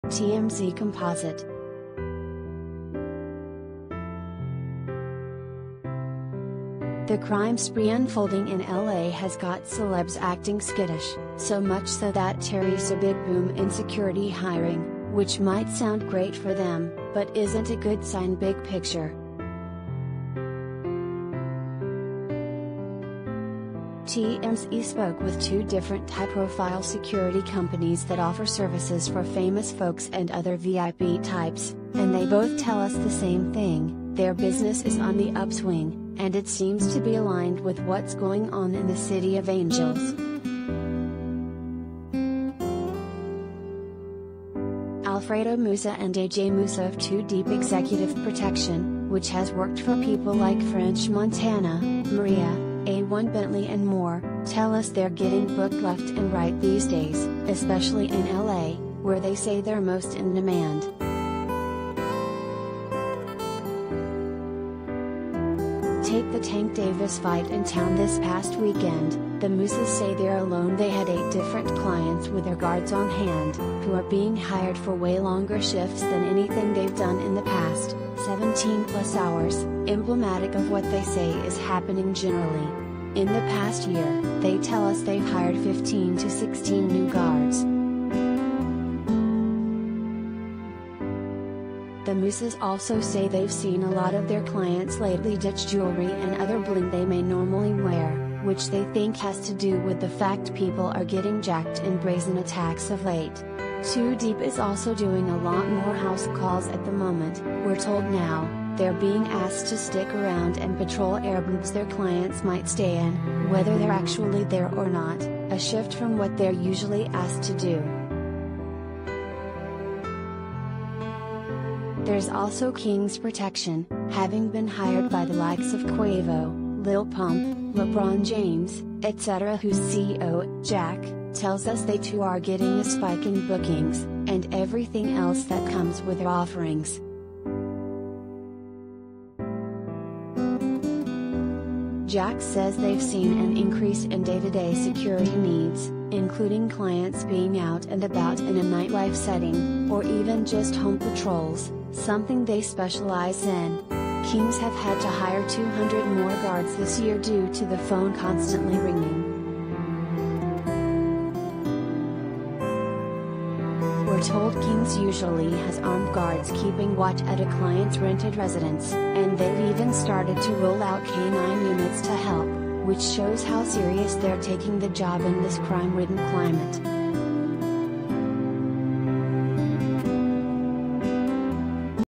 Security Companies Seeing Boom in Celeb Clientele Over Crime Spike
TMZ Composite
The crime spree unfolding in L.A. has got celebs acting skittish, so much so that there's a big boom in security hiring ... which might sound great for them, but isn't a good sign big picture.
TMZ spoke with two different high-profile security companies that offer services for famo